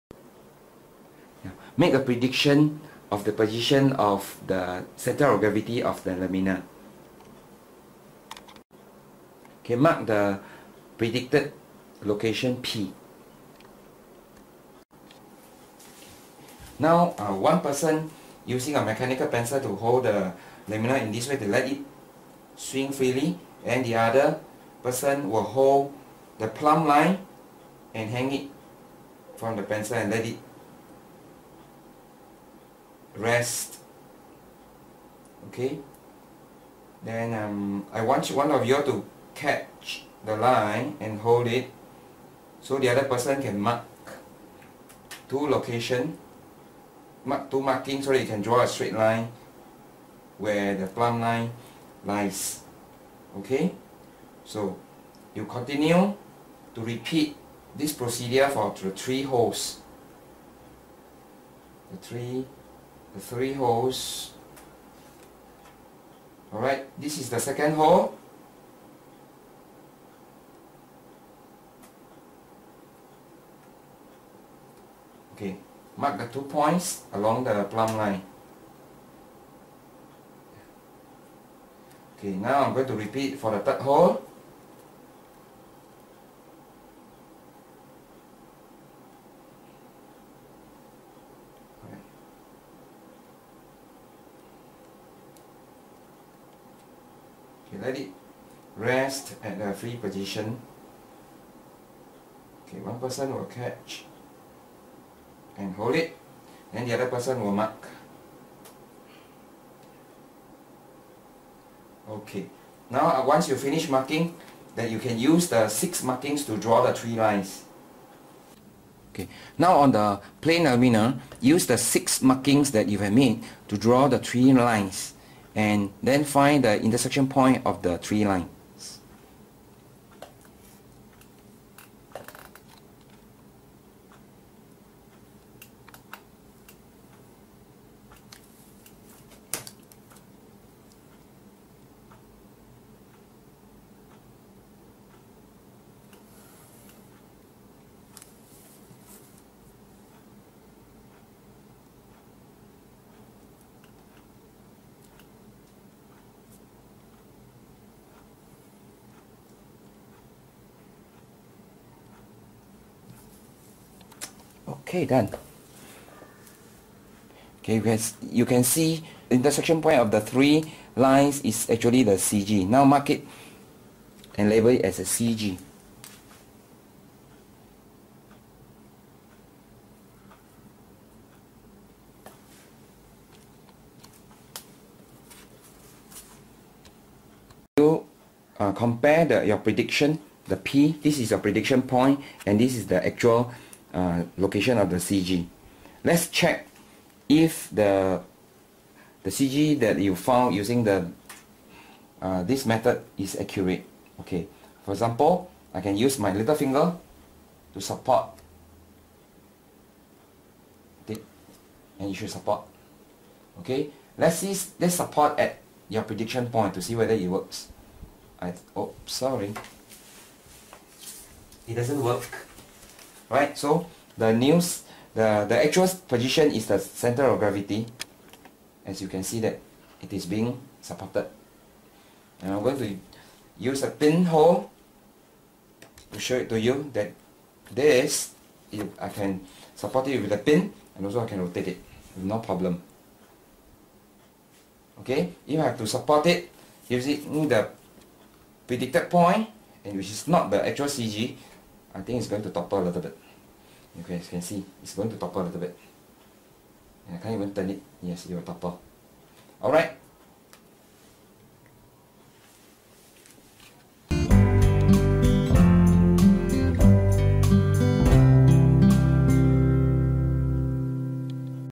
How to find the Centre of Gravity of a plane lamina hands on instruction
How to find the Centre of Gravity of a plane lamina hands on instructions. Step by step. You can do it with a friend without the need to use a laboratory setup!